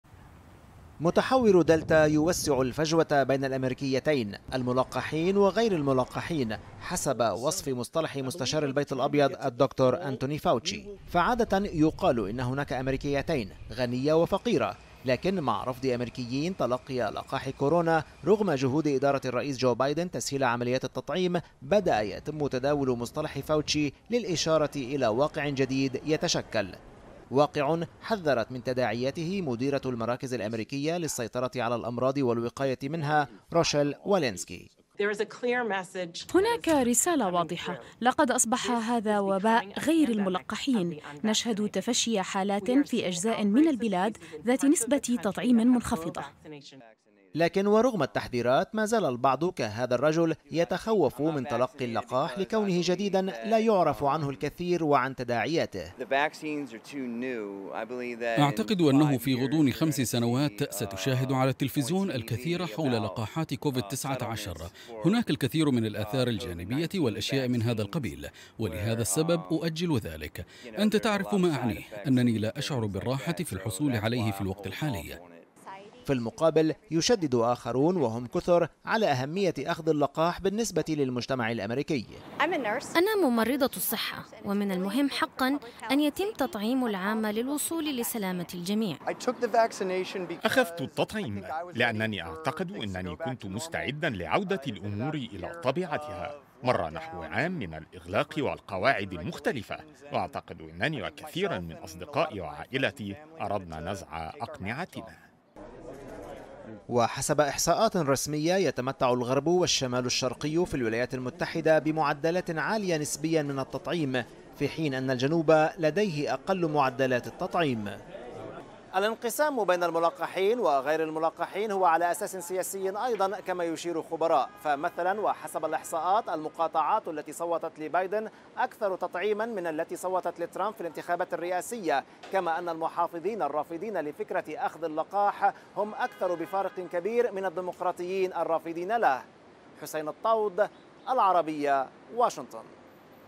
ما علاقة بايدن وترمب بالتطعيم في أميركا؟
متحور دلتا يوسع الفجوة بين الأميركيين.. لماذا يخشى بعض الأميركيين من التطعيم.. وما علاقة بايدن وترمب؟